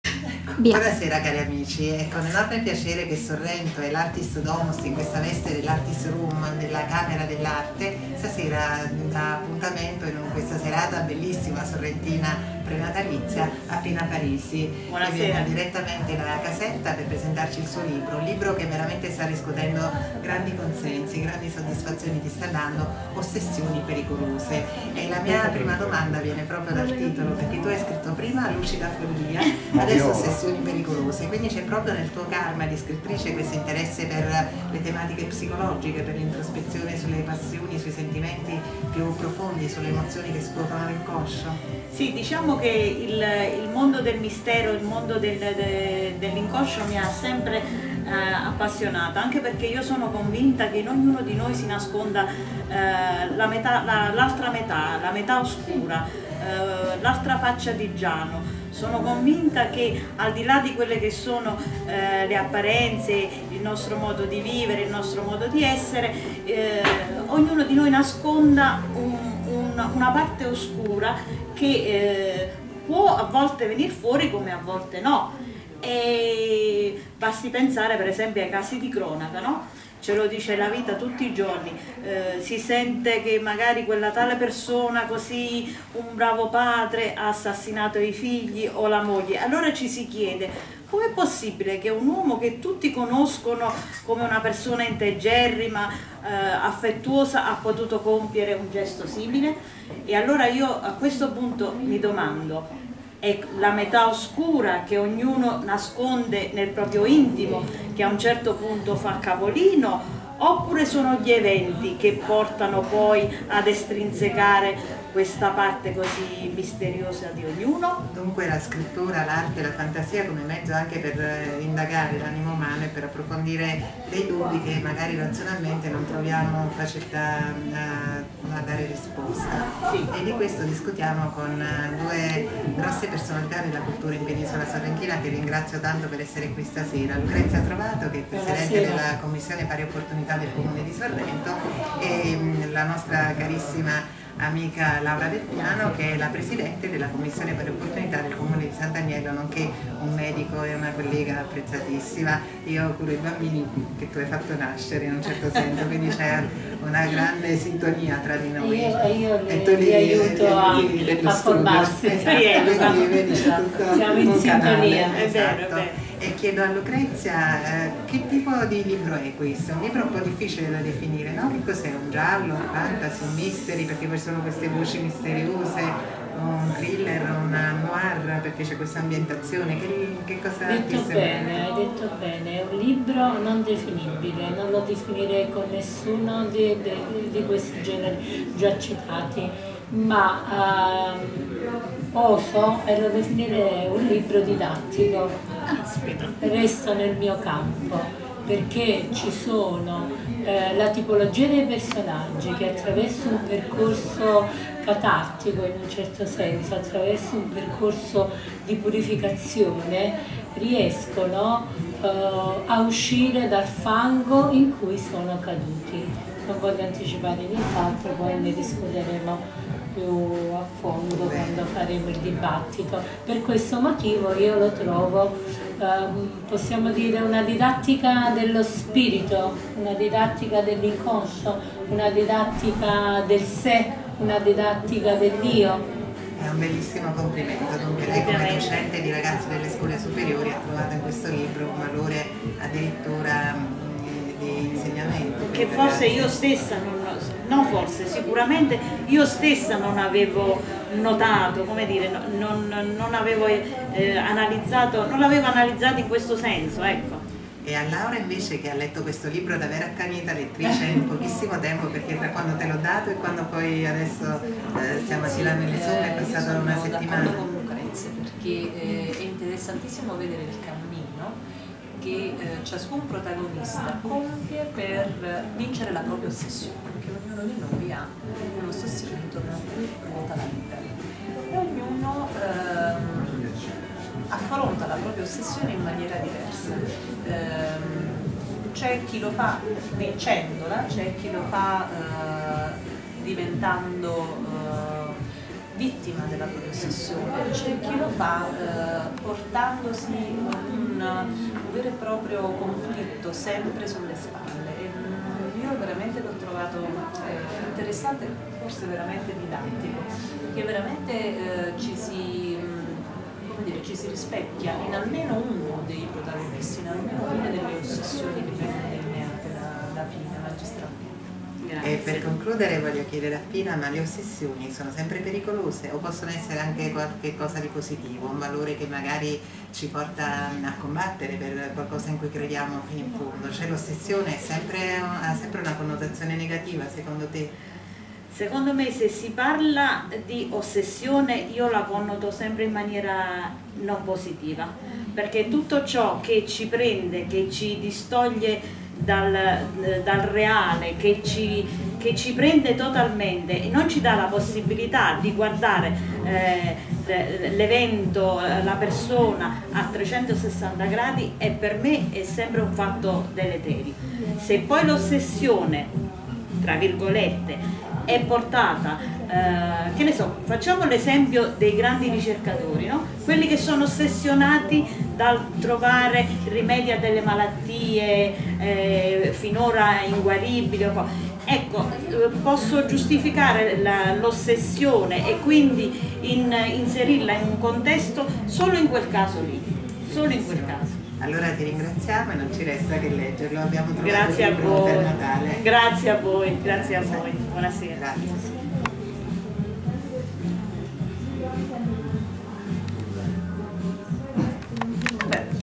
Pina Parisi con Lucrezia Trovato e Laura del Piano all'Artis Domus Sorrento
Per la rassegna cine-letteraria del Circolo Endas Penisola sorrentina Onlus, Carlo Alfaro ha ospitato all'Artis Domus Sorrento il 7-12-14 nel salotto letterario "All'ombra del Tasso" la scrittrice Pina Parisi con il suo appassionante thriller “Ossessioni pericolose”, commentato dalla professoressa Lucrezia Trovato e la dottoressa Laura Del Piano, Presidenti rispettivamente delle commissioni Pari opportunità di Sorrento e Sant'Agnello. Riprese a cura di Lia Trapani.